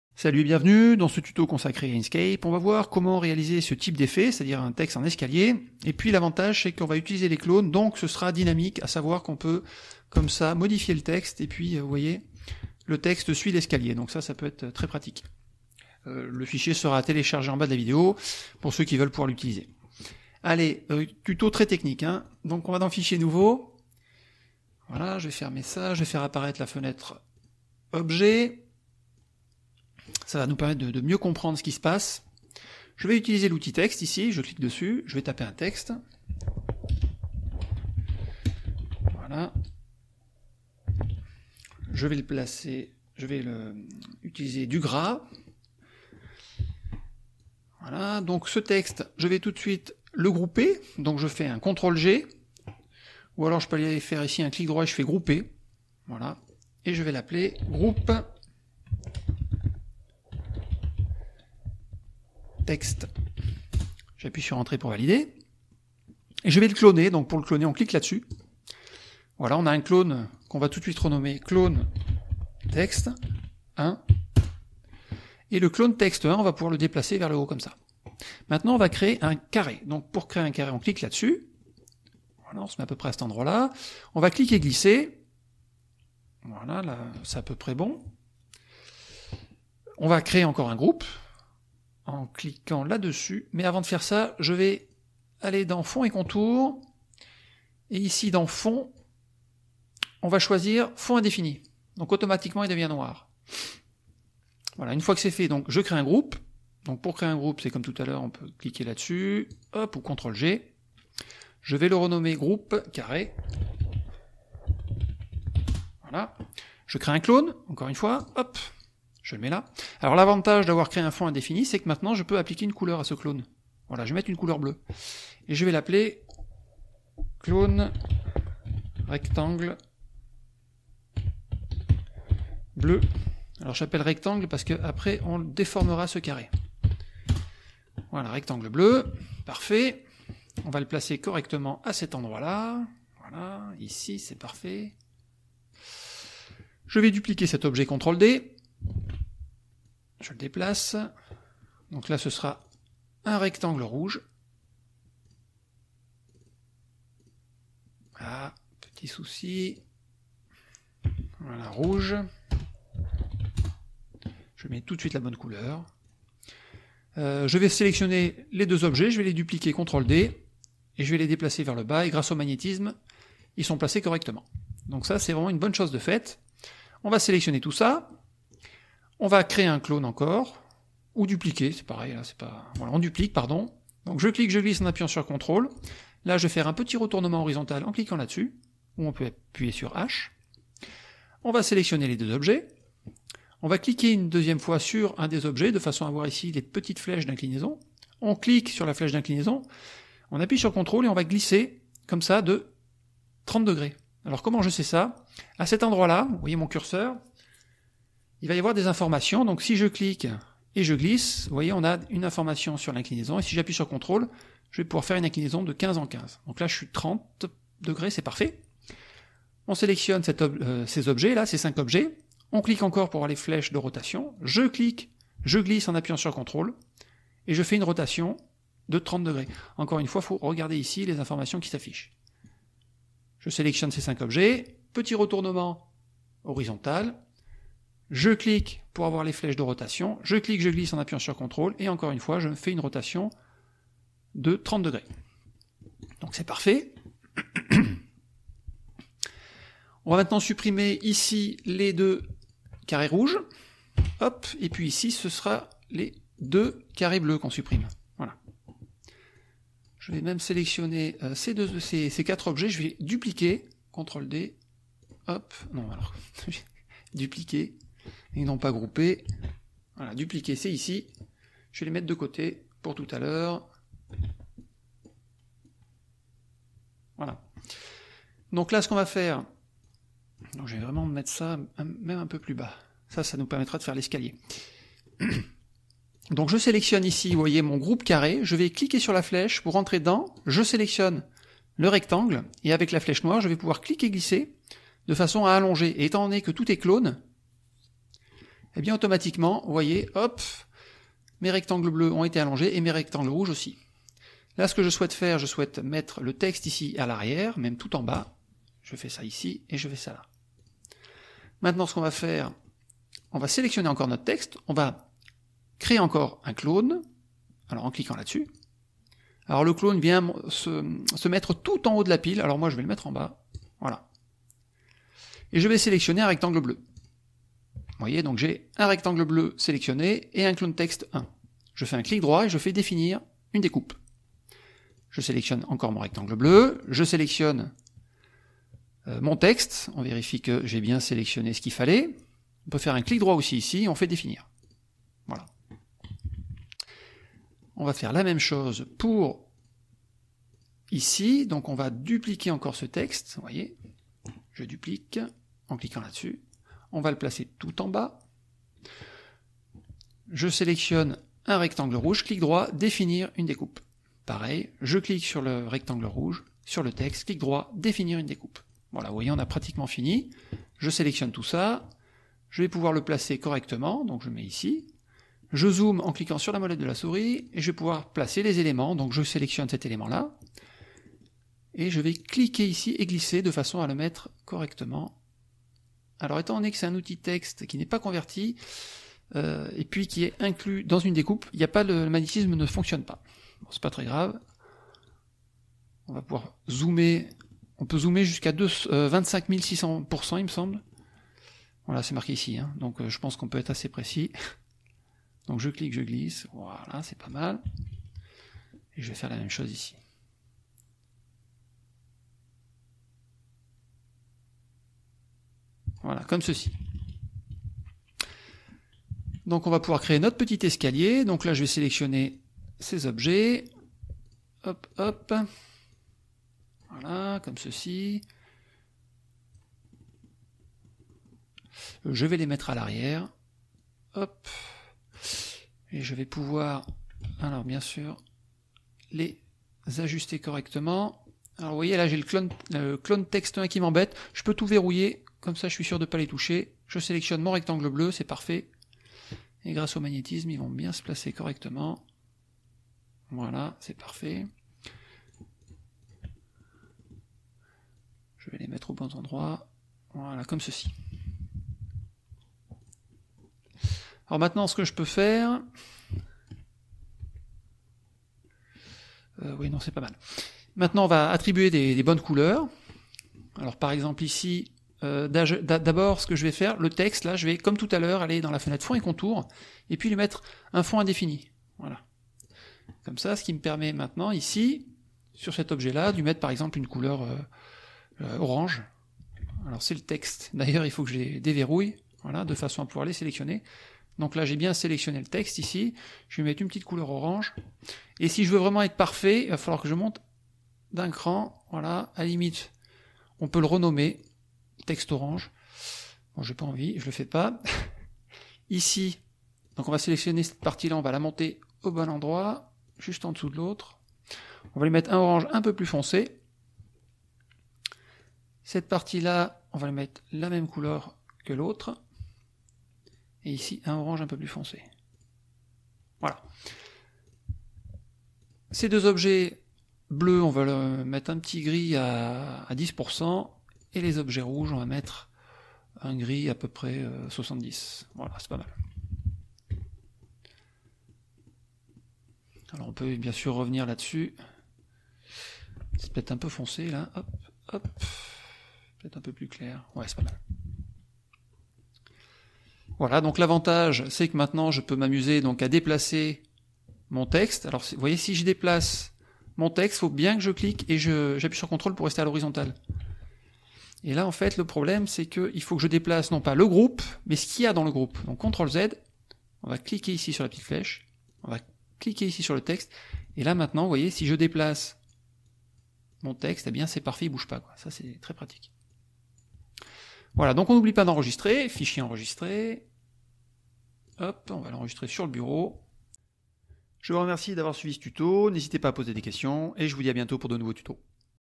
Tuto inkscape : Effets de texte " escalier " (épisode n°4 - expert)
Nous allons découvrir comment faire un effet de texte en " escalier "